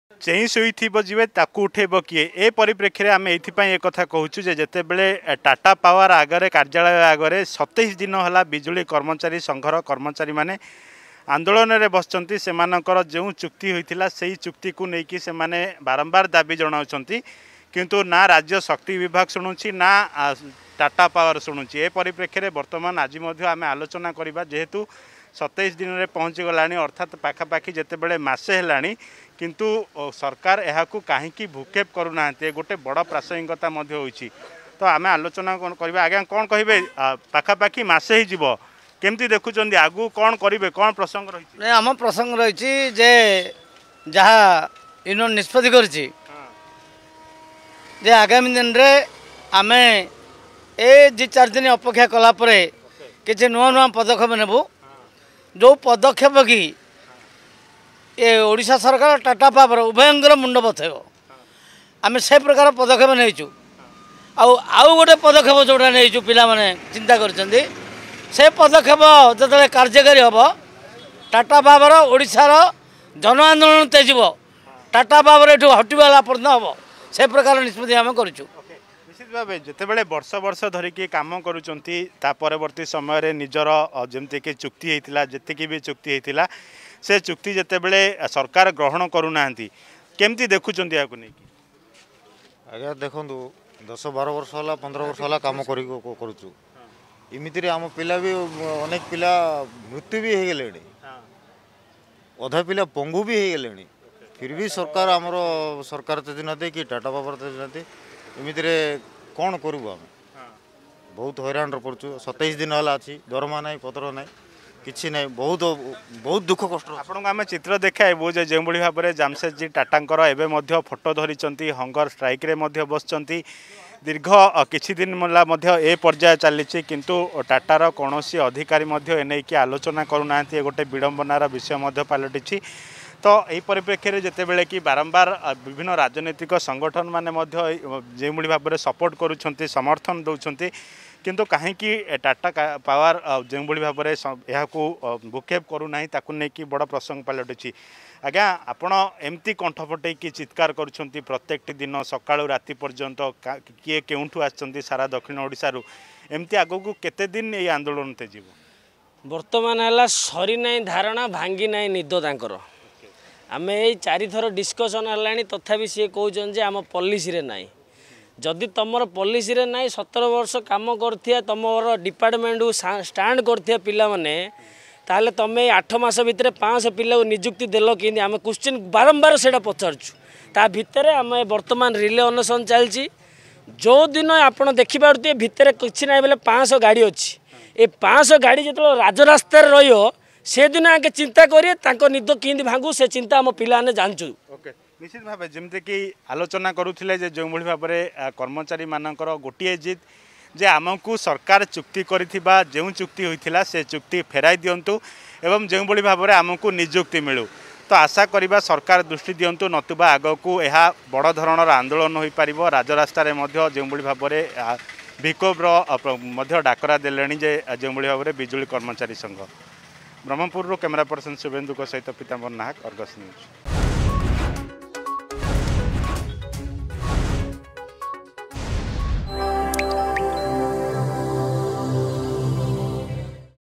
ସରକାର ଏବଂ ଟାଟା ପାୱାର ଉଭୟ ନିଦାବିଷ୍ଣୁ
27 ଦିନହେଲା ରାଜରାସ୍ତାରେ ଶହ ଶହ ଆଉଟସୋର୍ସ ବିଜୁଳି କର୍ମଚାରୀ । ସରକାର ଏବଂ ଟାଟା ପାୱାର ଉଭୟ ନିଦାବିଷ୍ଣୁ